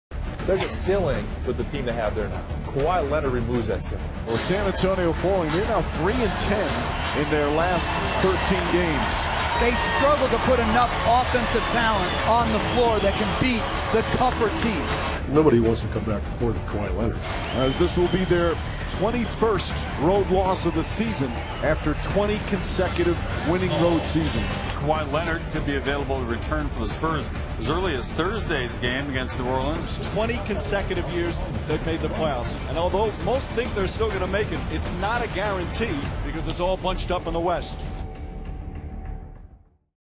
The San Antonio Spurs are in desperate need of Kawhi Leonard | ESPN
The San Antonio Spurs had historic struggles in February 2018 and hope to turn things around with Kawhi Leonard's expected return.